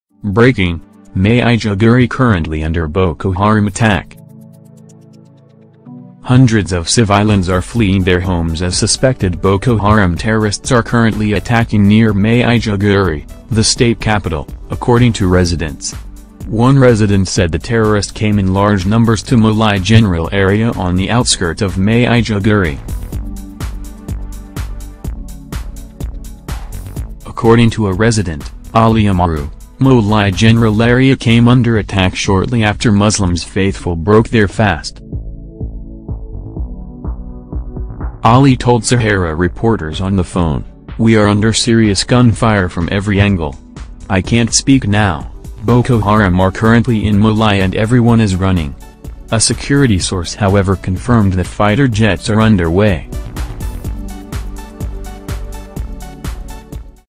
BREAKING: Maiduguri Currently Under Boko Haram Attack
Ali told SaharaReporters on the phone, "We are under serious gunfire from every angle.I cant speak now, Boko haram are currently in Molai everyone is runing."